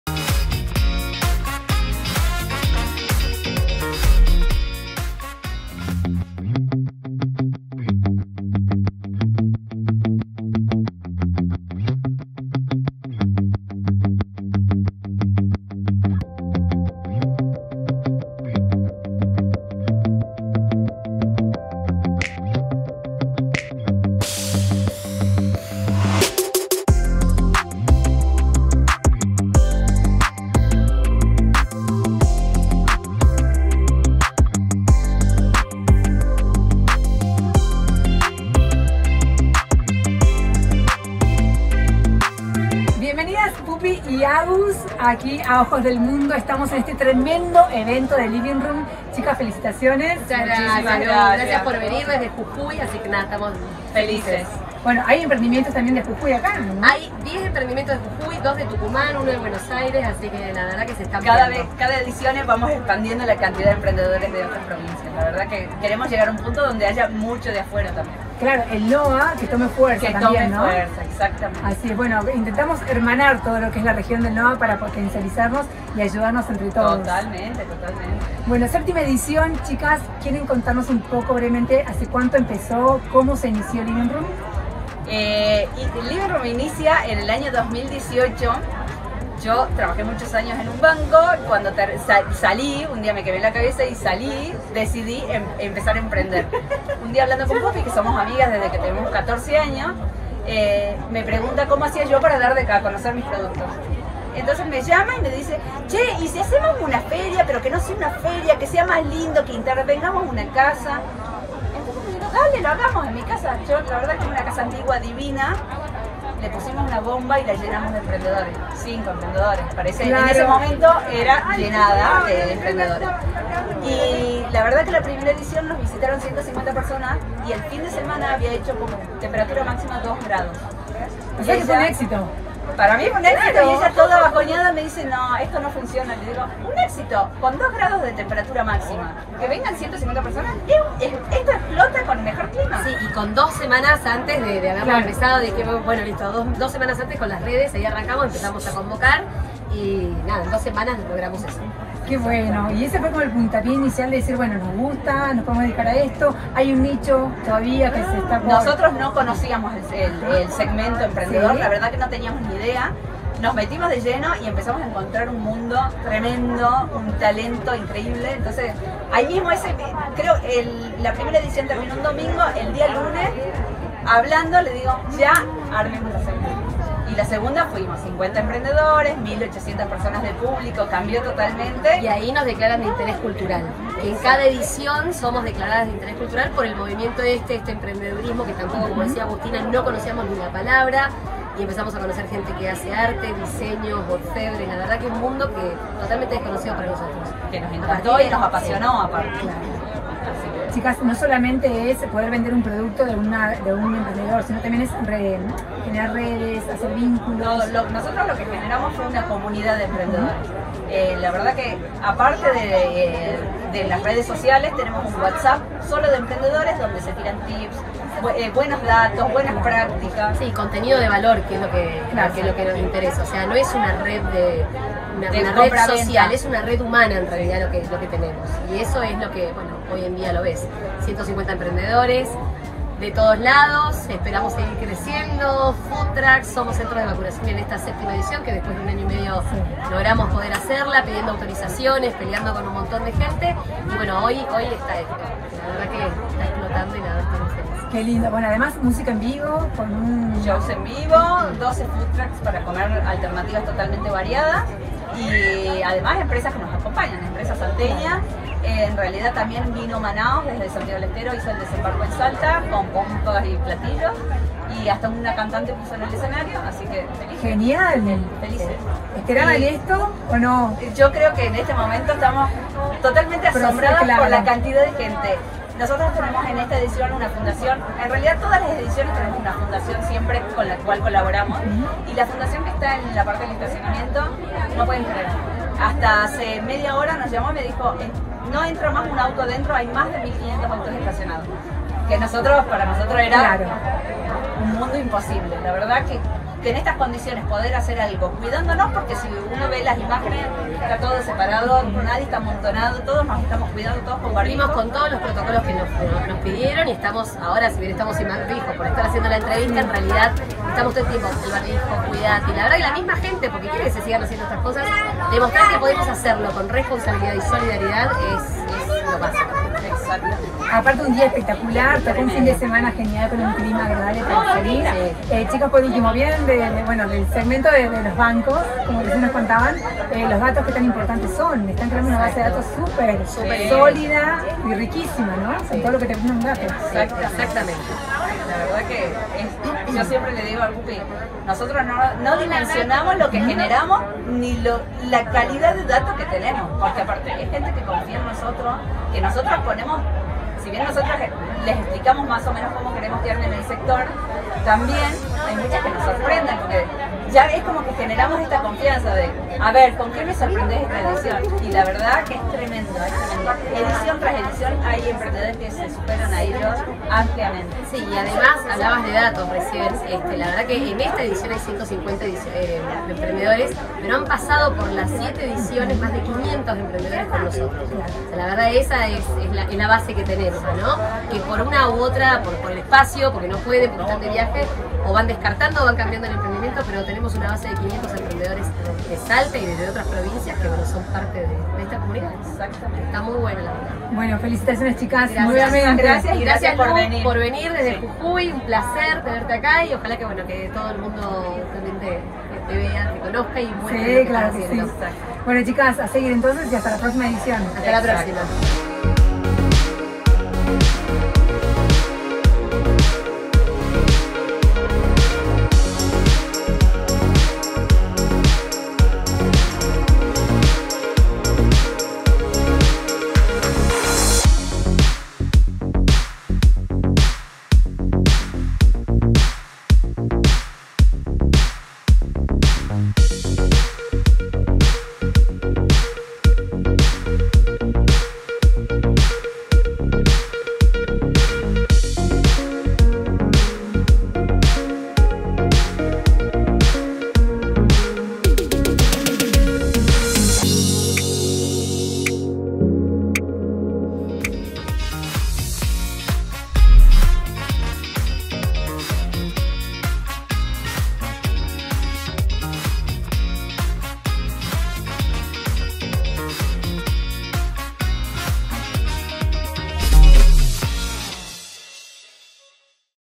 Visitamos la feria Livingroom en Salta
Conozcamos el espíritu emprendedor de Pupy y Agus las creadoras de Livingroom una comunidad de emprendedoras.